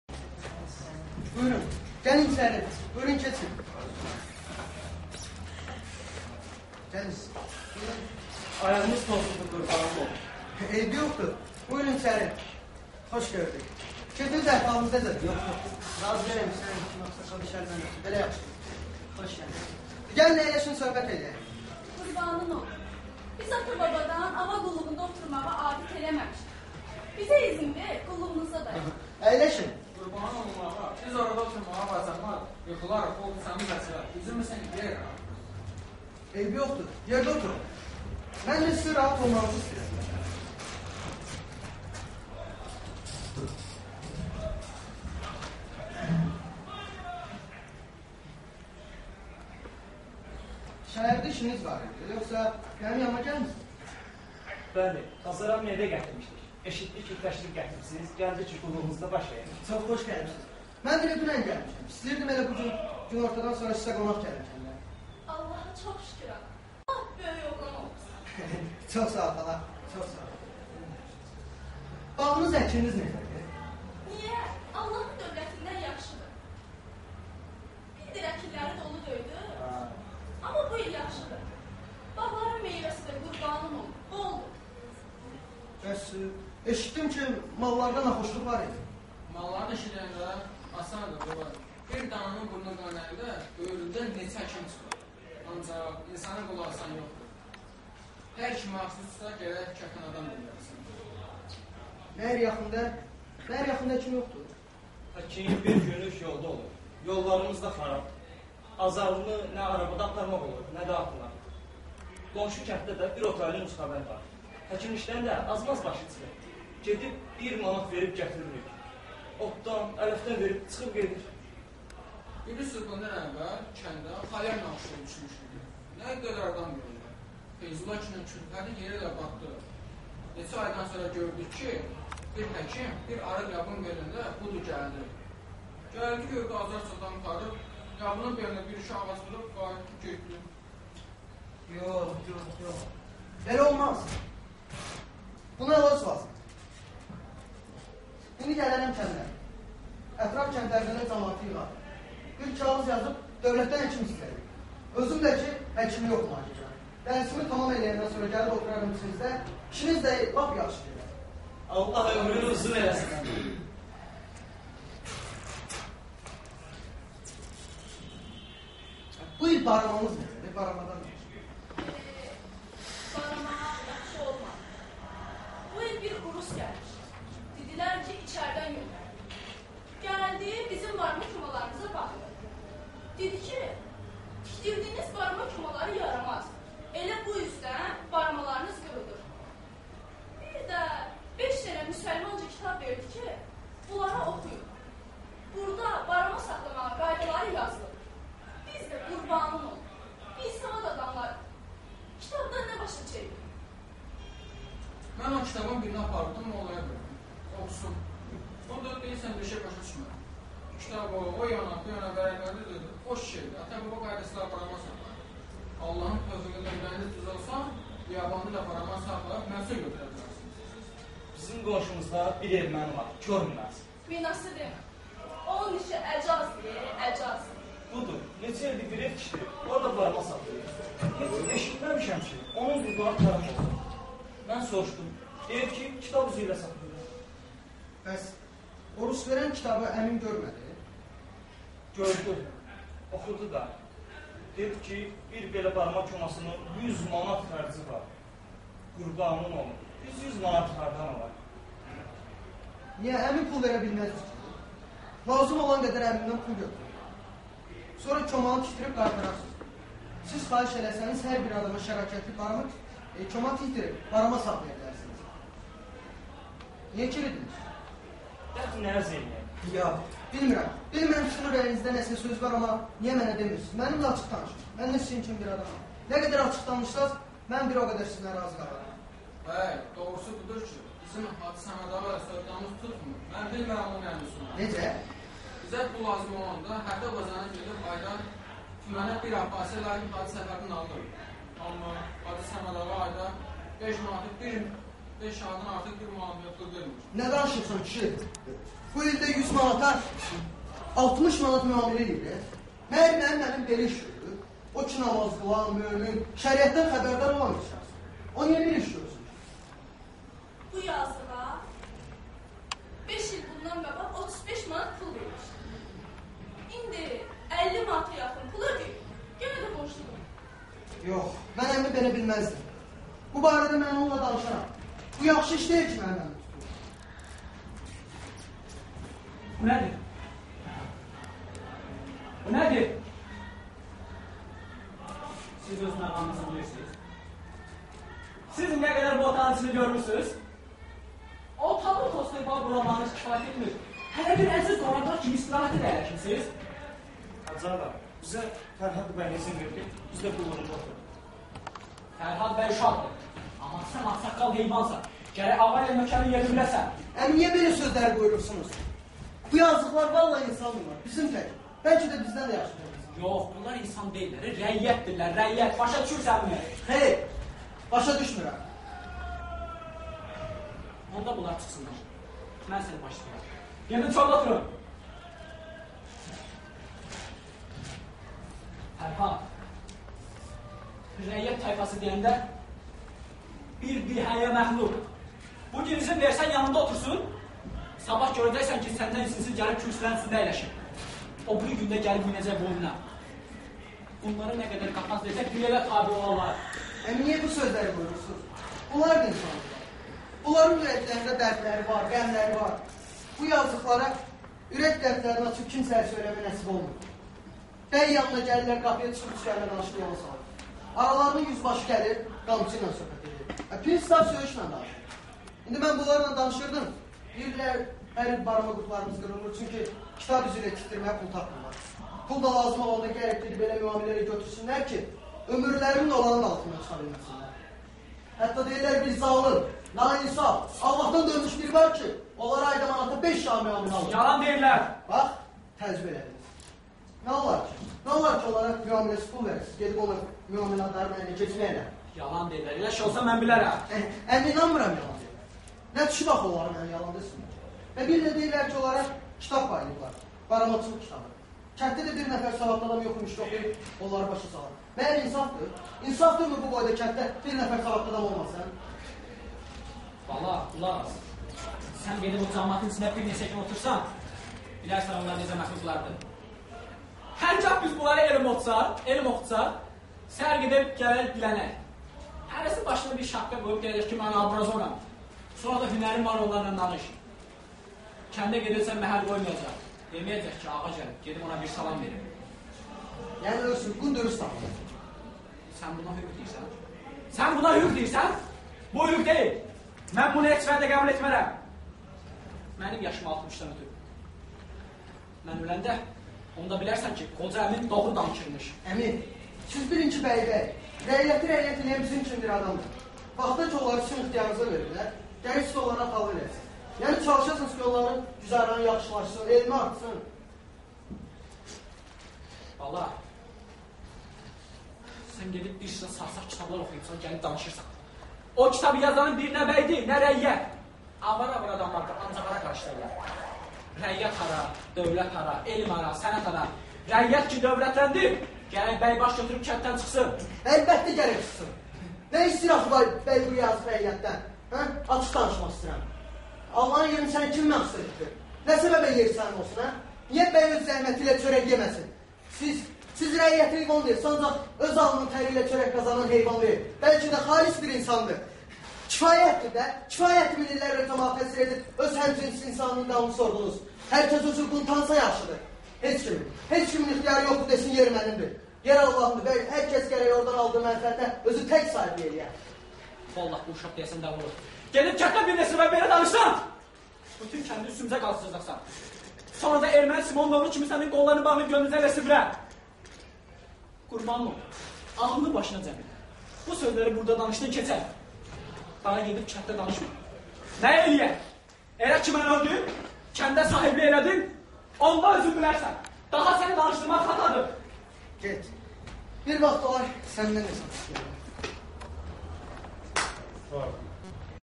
Ə.B.Haqverdiyev - Bəxtsiz cavan (bir parça)
Azərbaycan Dövlət Mədəniyyət və İncəsənət universiteti
I kurs "dram rejissorları"
Tural Ələkbərov
Aysel Hacıyeva
Emil Hacıyev
Cavid Yusifov
Rafael Muradxanlı